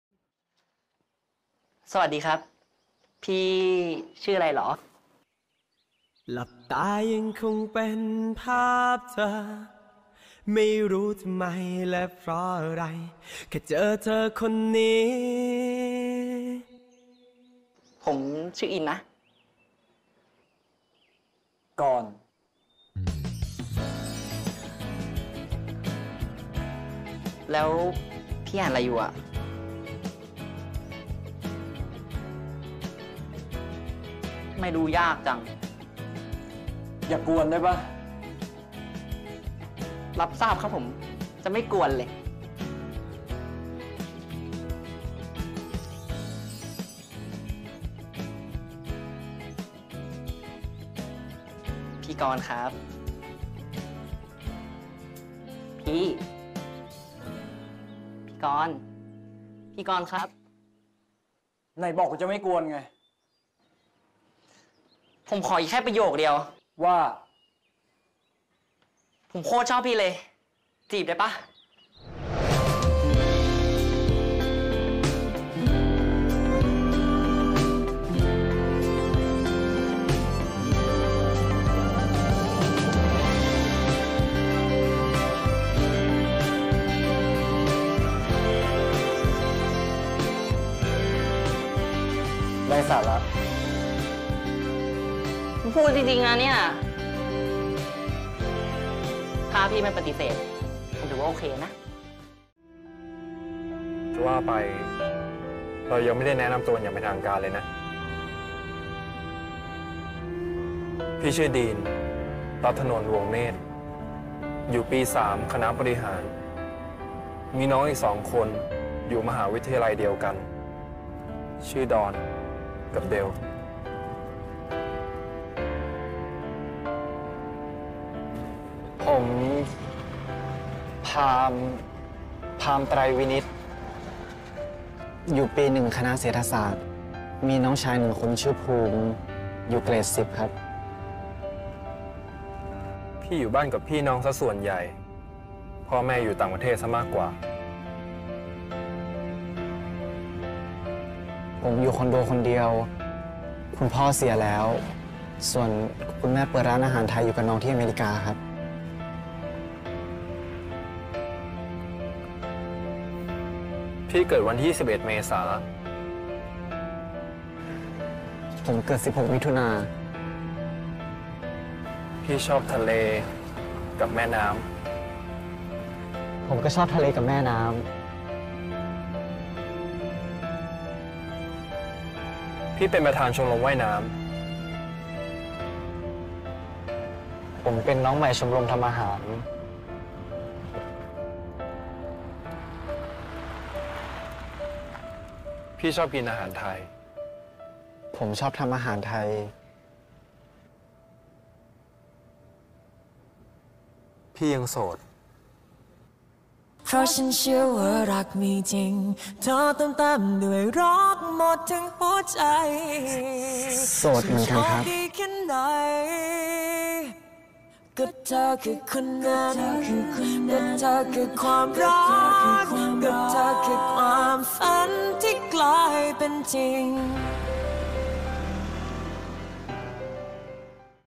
Introducing yourself 'KornIn version vs DeanPharm version
I just found these scenes amusing, adorable and cute 😍😍😍 their getting to know each other is quite unusual and unique 😁😁